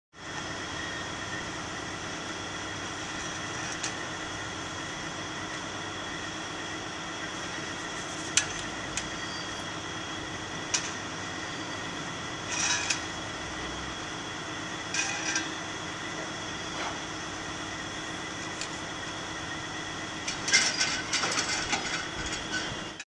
Adding Edge Protectors for Flat Bed Truckers - Made in the USA
The COBRA® Strap Handler launches heavy tie-down straps effortlessly, over your high loads. This patented heavy duty aluminum telescoping rod and precision strap housing gives you the height for perfect strap positioning every time.

No more twisted straps. No more misses. No more re-rolling straps for a second shot. How much time do YOU waste tossing straps around? End the pain in your arms and shoulders from throwing straps over your flatbed.

Made in the USA.  Developed by a Flat Bed Trucker ... for Flat Bed Truckers